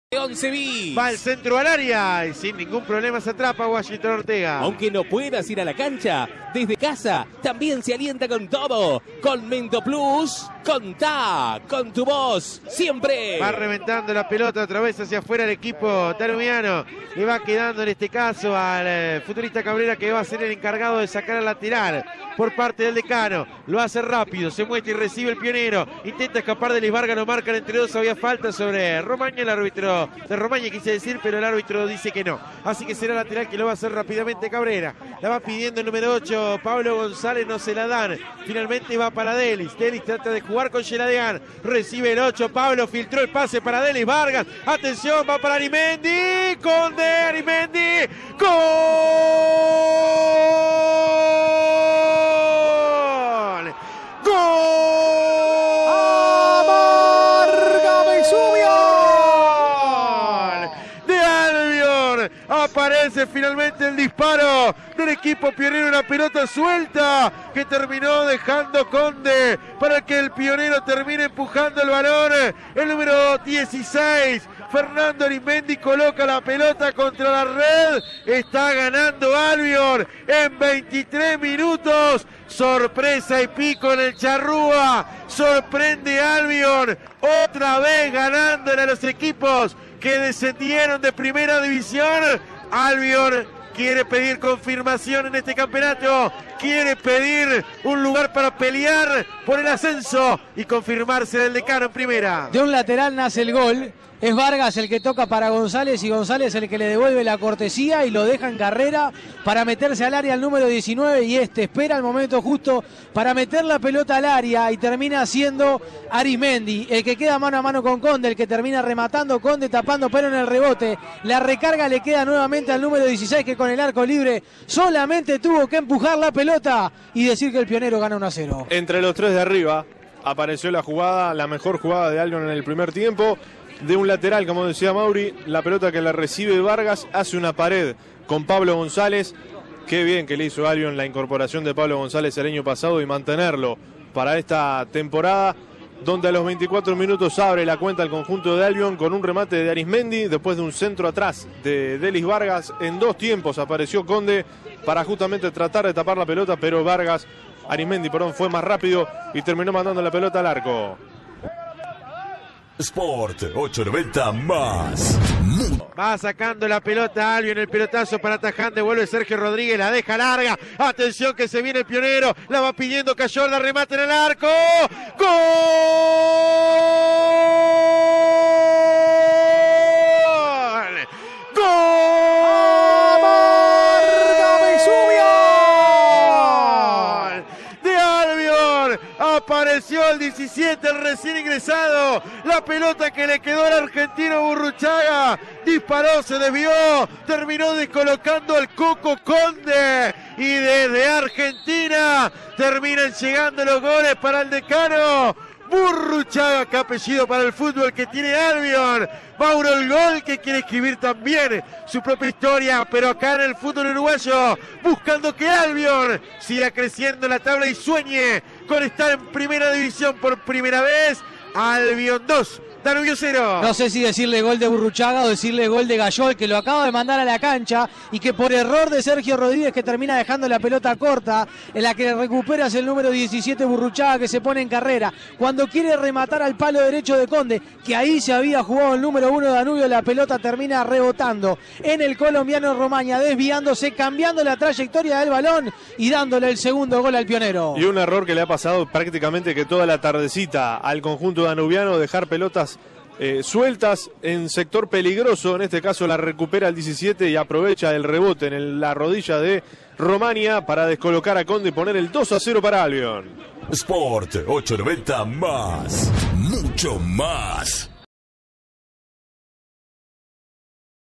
Albion 2 Danubio 0 - Segunda 2021 (por Nicolás Pirri)
Los goles del partido, por Nicolás Pirri junto a Mauricio Raggiotto y Santiago Pepe para CX 18 Radio Sport 890 AM (Señal Plus).